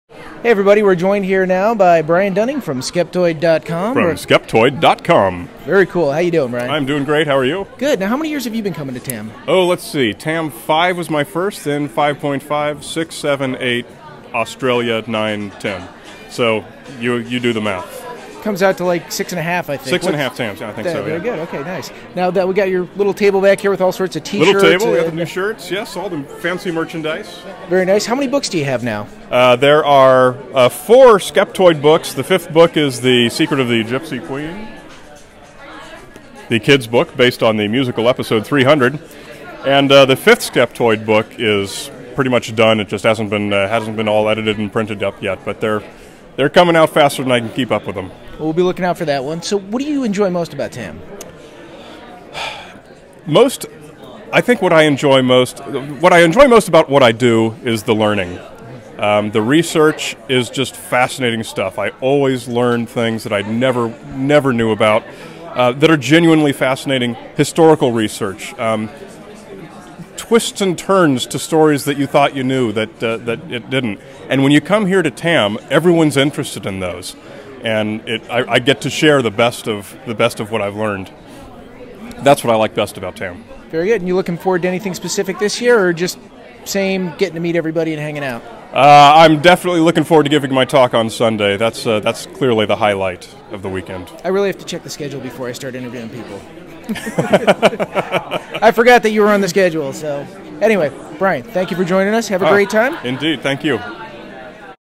The Amazing Meeting 2012: Brian Dunning
From The Amazing Meeting 2012

A quick chat with Christopher Brown of the "Meet The Skeptics" Podcast and Brian Dunning from Skeptoid.